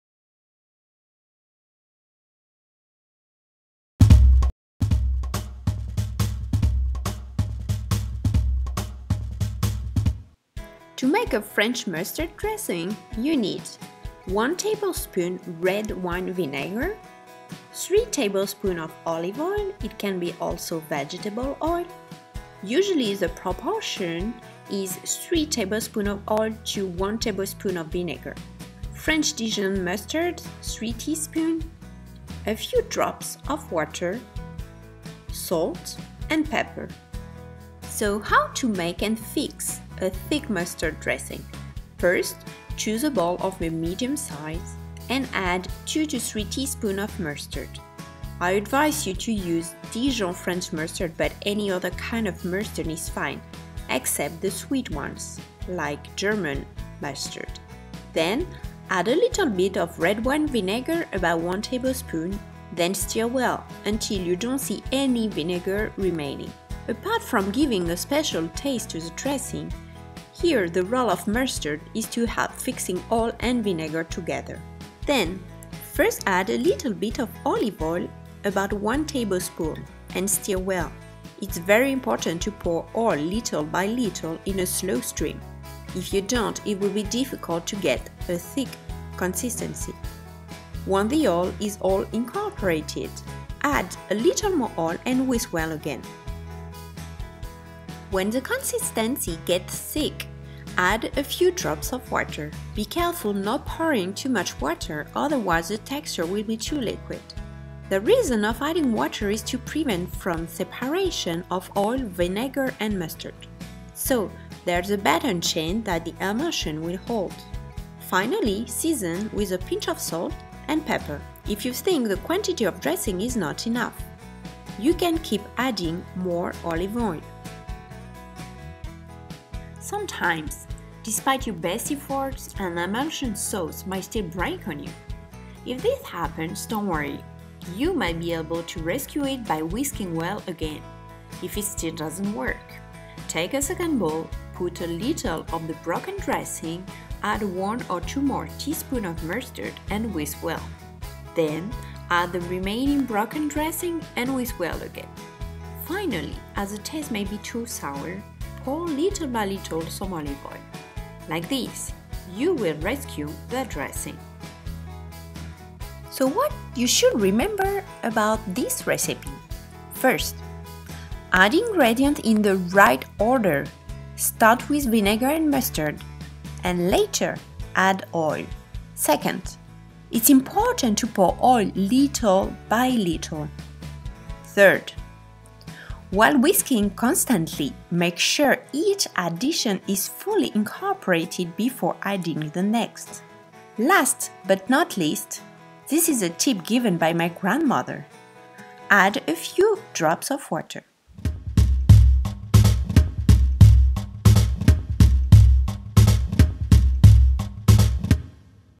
怎么做西式沙拉酱汁 ？想学做法国美食 ？ Recette de la sauce salade en chinois
想学做法国美食 ？
Recettes de cuisine française en chinois/mandarin
French recipes in Chinese